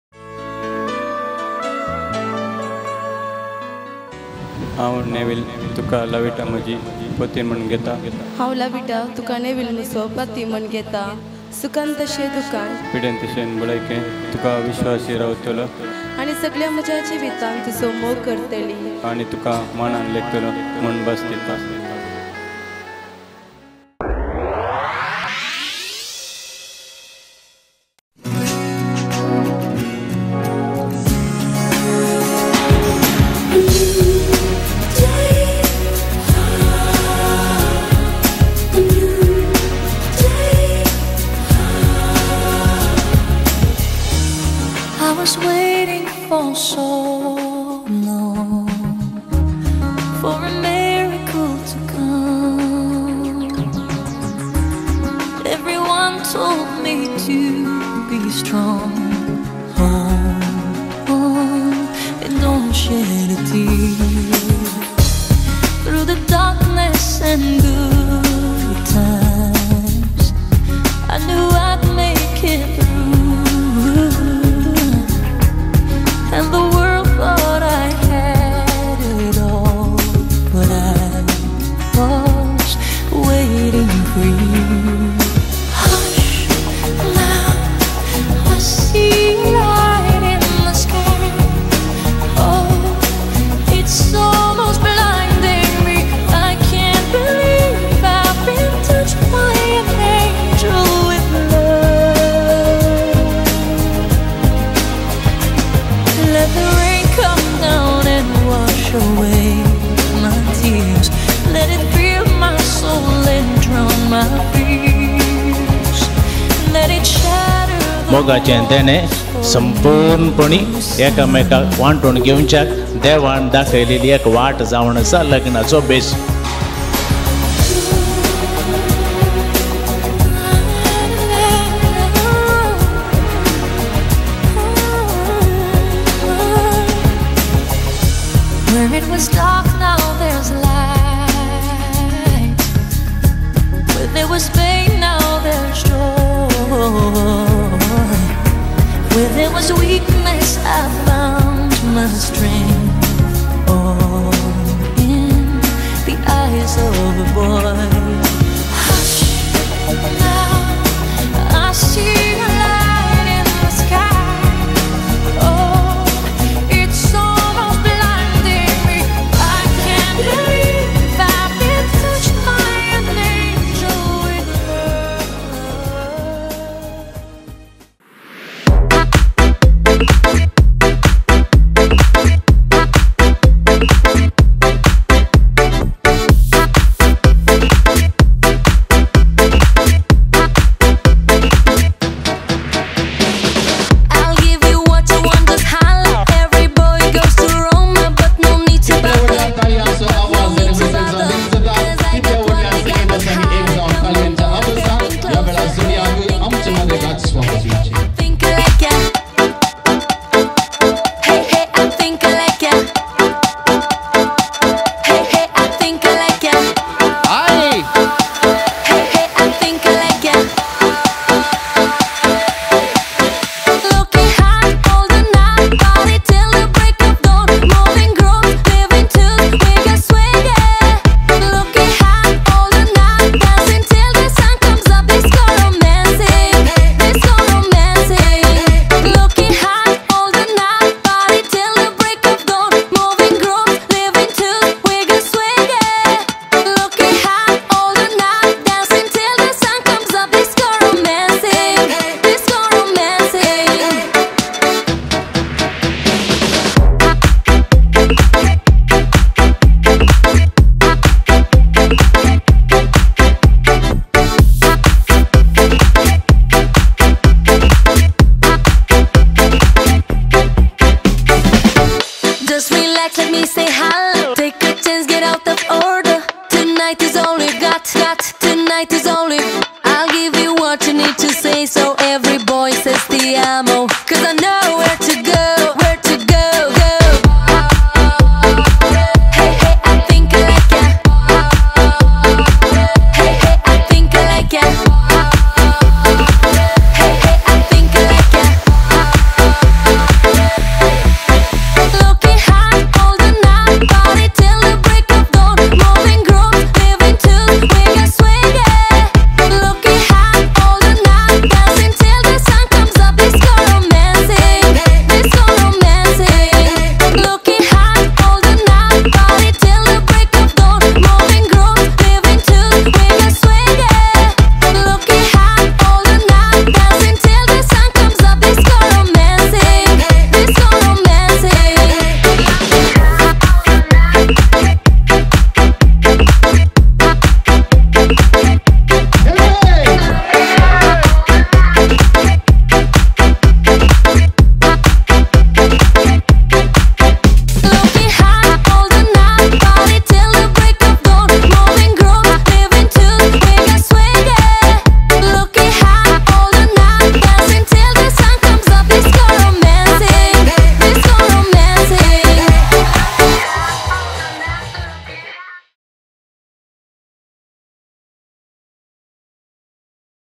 NEVIL-LAVITA Wedding Short Movie, Mangalorean Catholic Ceremony-Highlights by Nelson Photography
NEVIL - LAVITA

Video Highlights

Wedding Ceremony  Short Movie

Videography by: Nelson Photography,

Bantwal - Mangalore.

The original Copyright of the songs & Music solely owned by the Companies/ Original-Artists